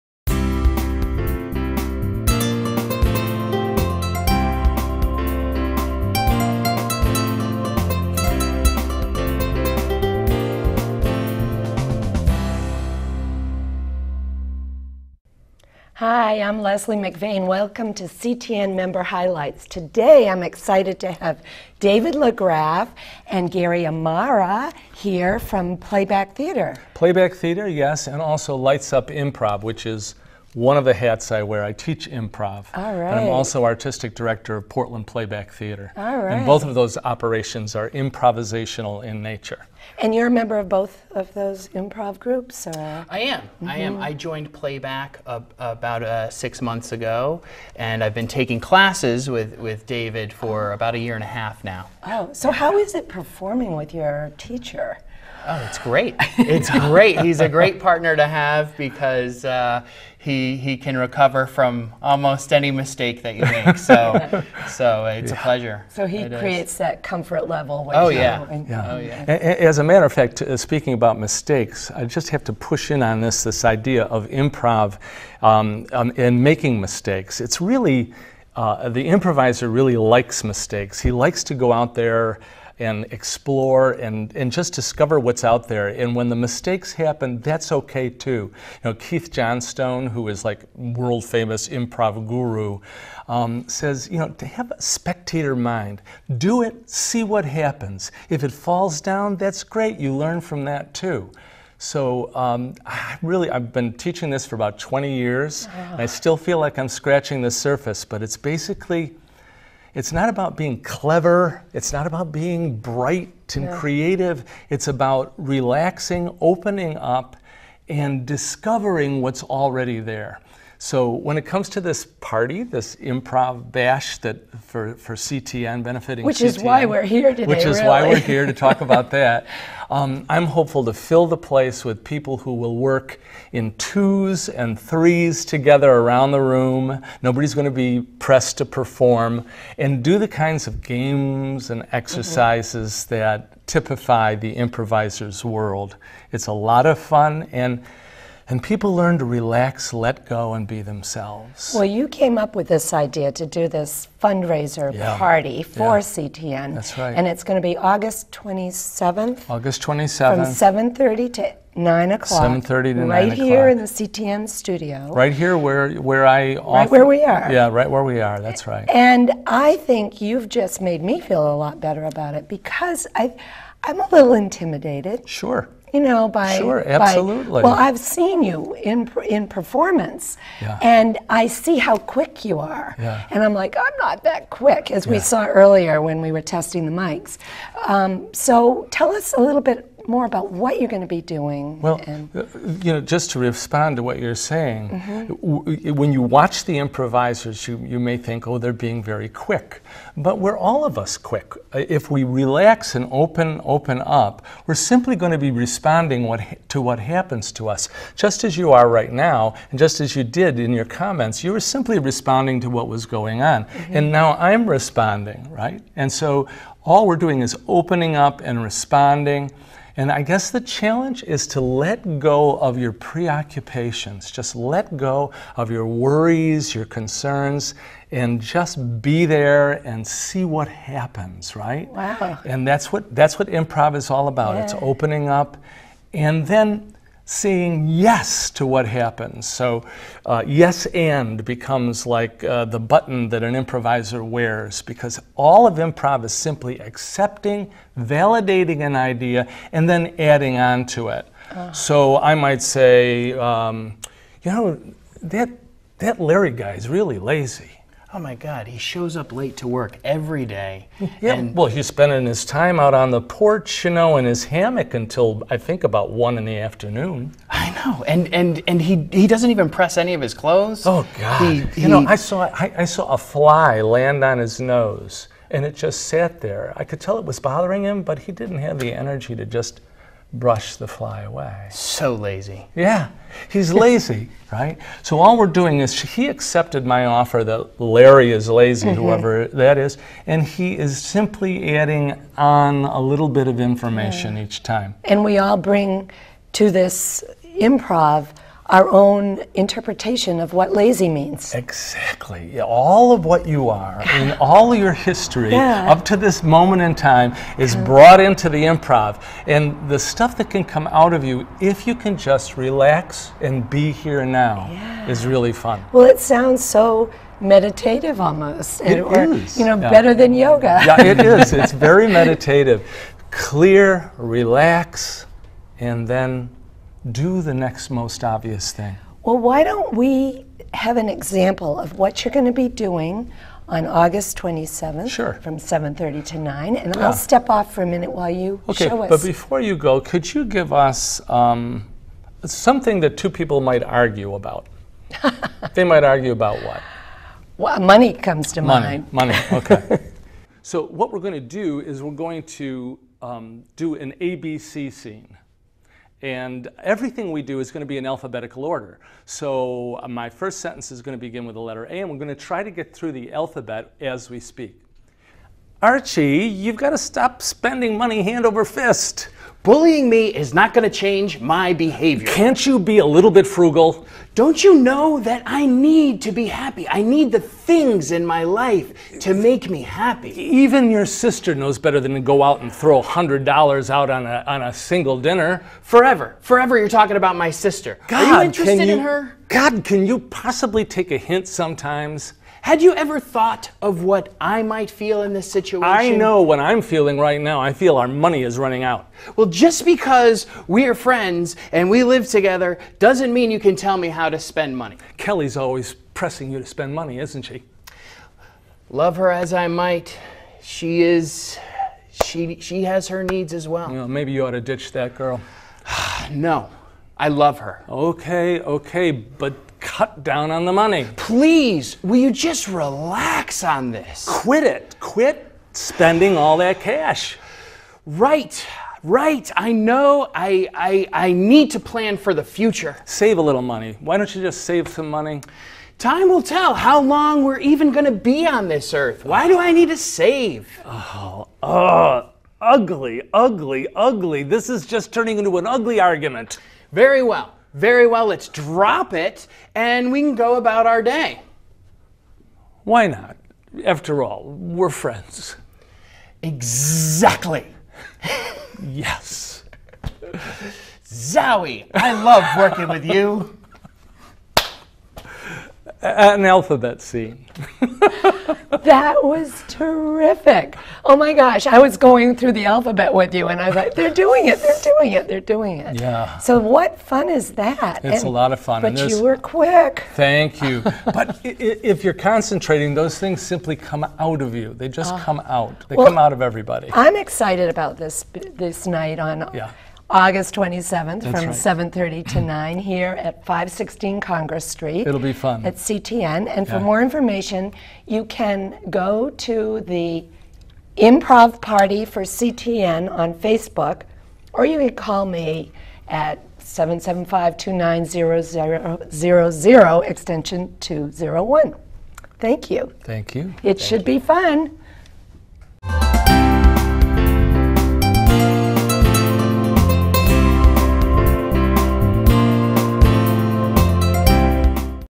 Portland Playback CTN Fundraiser
Find out about Portland Playback Theatre's upcoming fundraiser for CTN 5